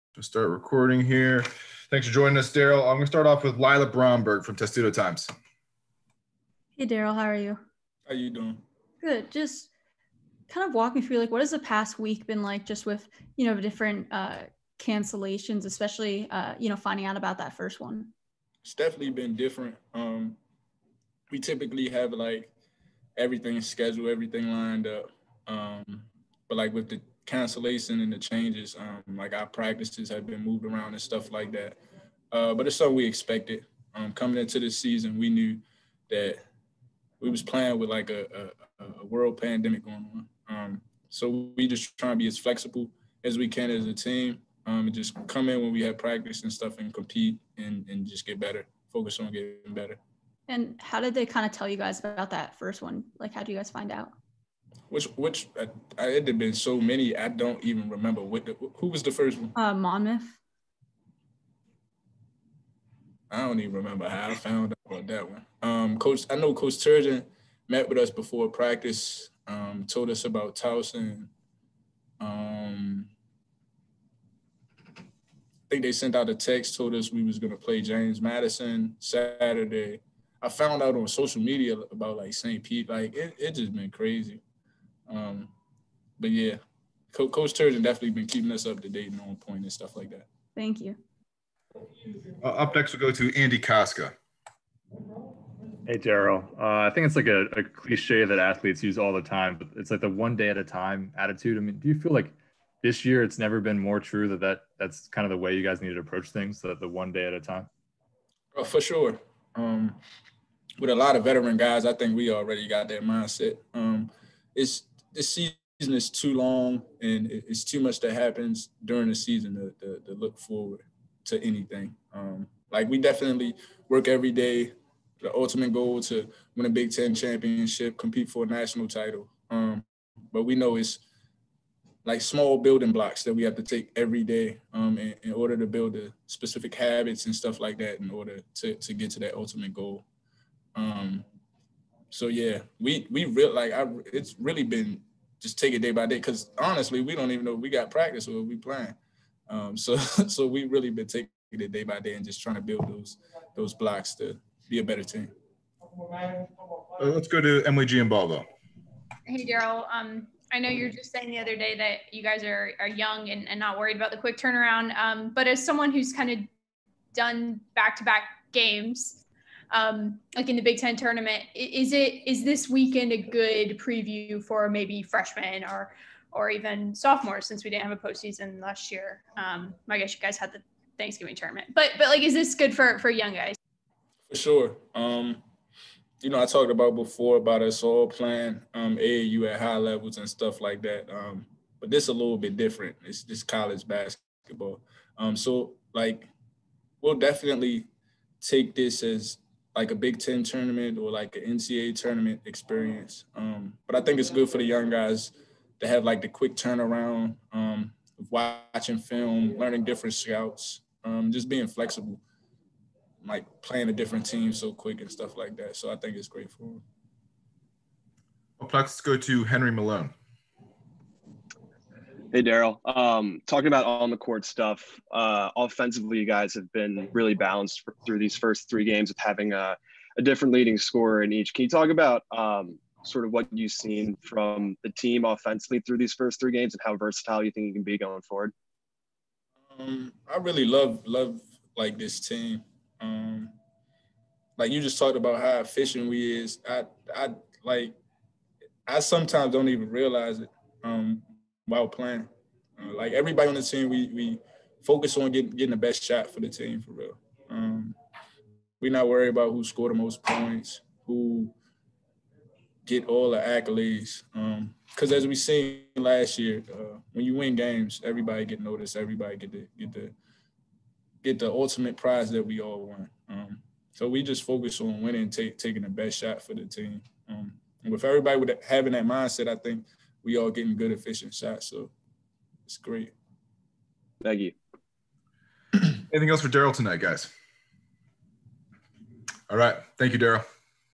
Darryl Morsell - Saint Peter's pregame
Senior guard Darryl Morsell previews the Terps' next home game versus Saint Peter's.